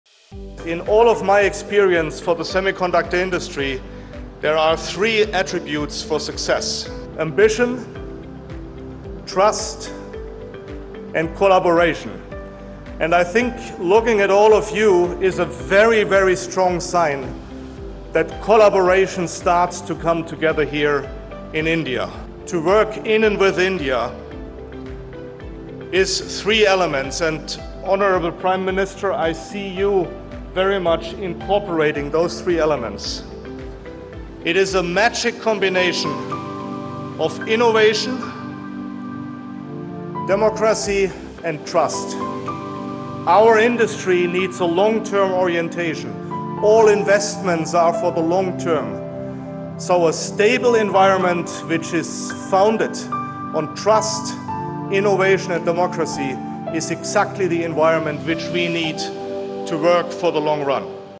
NXP CEO applauds PM Narendra Modi's magic combination of innovation, democracy & trust!
Kurt Sievers, CEO of NXP lauded PM Modi for what he called a "magic combination" of innovation, democracy, and trust. In his remarks, the CEO emphasized how PM Modi's leadership has fostered an environment where cutting-edge technology thrives.